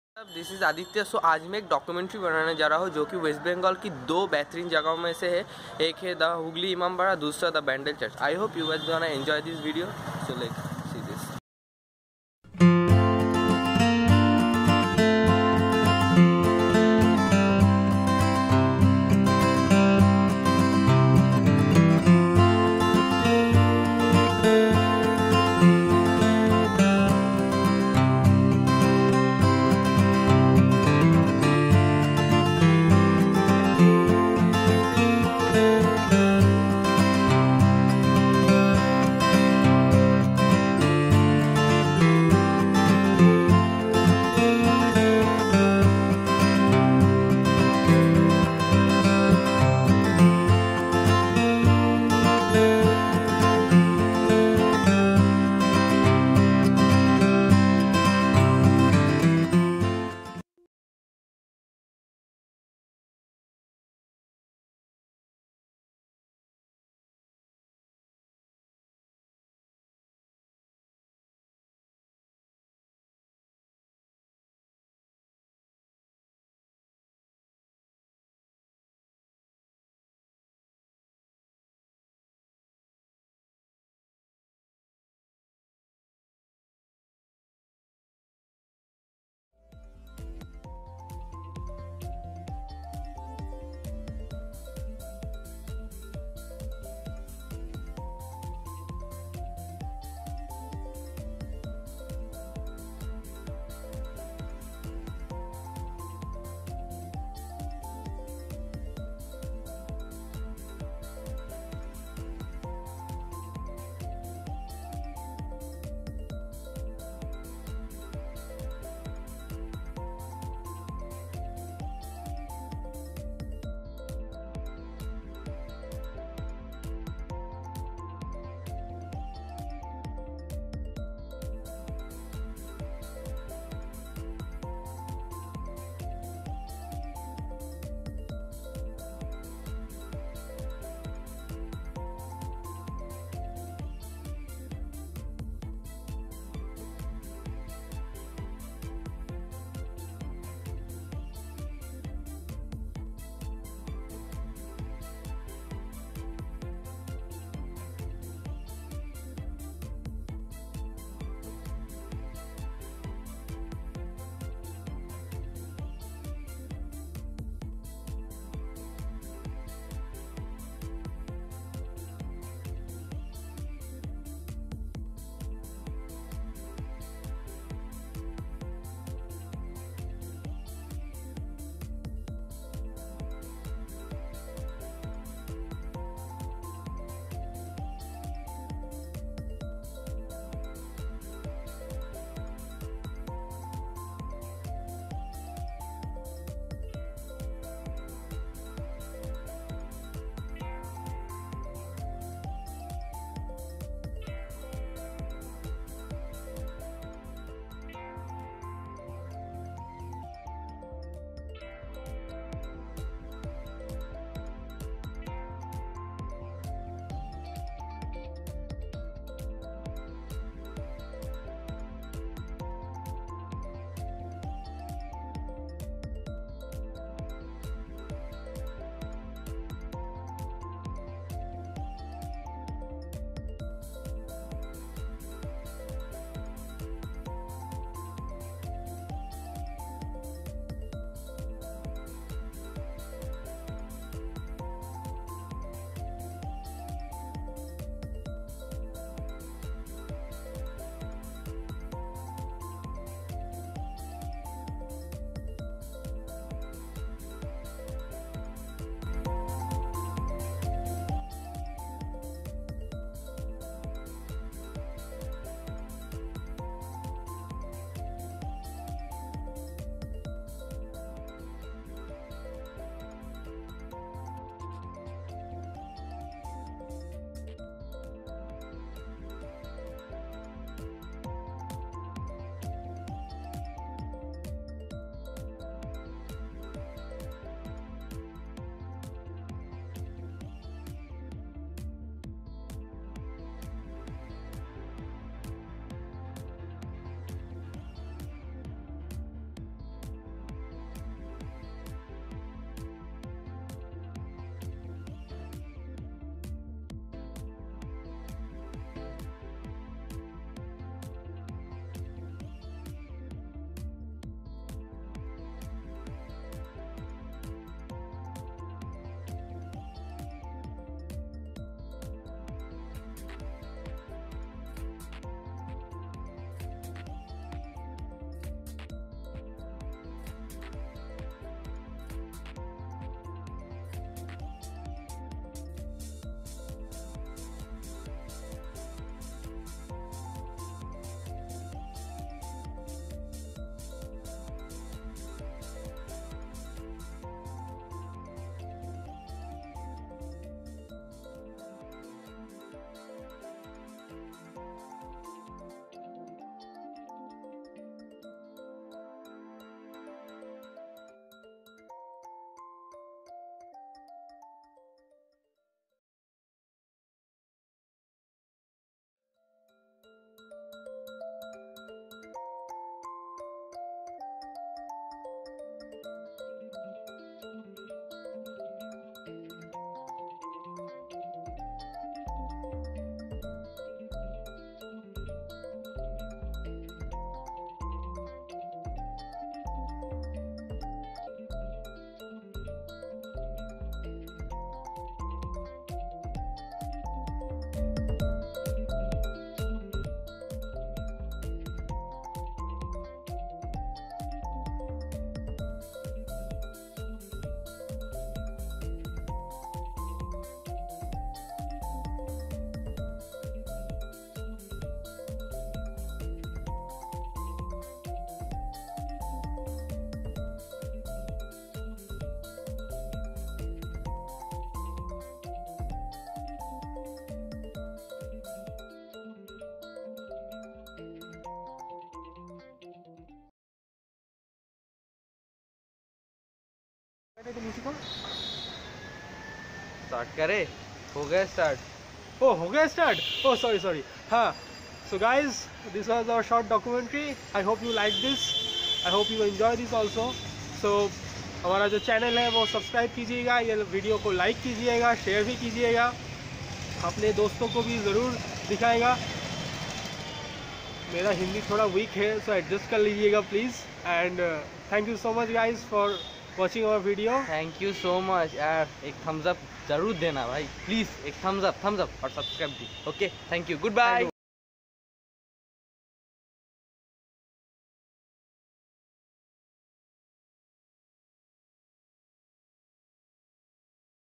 Short documentary of three culture( Imambara , Bandel church , Hanseswari temple )
a short trip to Imambara , Bandel church and Hanseswari temple. ita a documentary about West Bengal"s three tourist spotand most importantly its a short documentary about three culture.